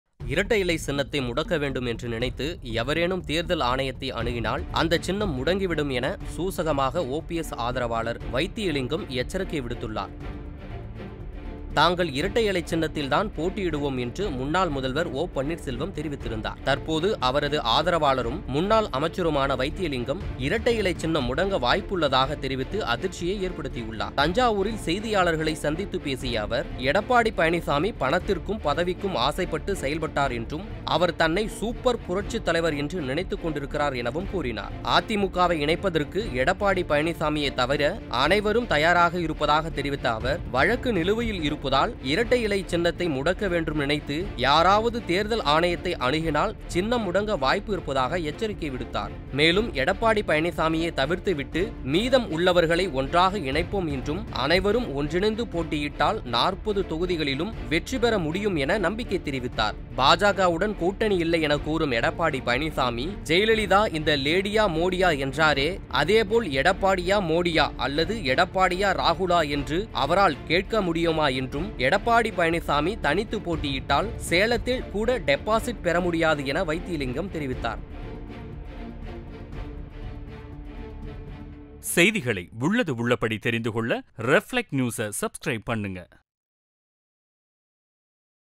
இரட்டை இலை சின்னம் விரைவில் முடக்கம்? அதிர்ச்சி கொடுத்த முன்னாள் அமைச்சர் | OPS Vs EPS
இரட்டை இலை சின்னம் விரைவில் முடக்கம்? அதிர்ச்சி கொடுத்த முன்னாள் அமைச்சர்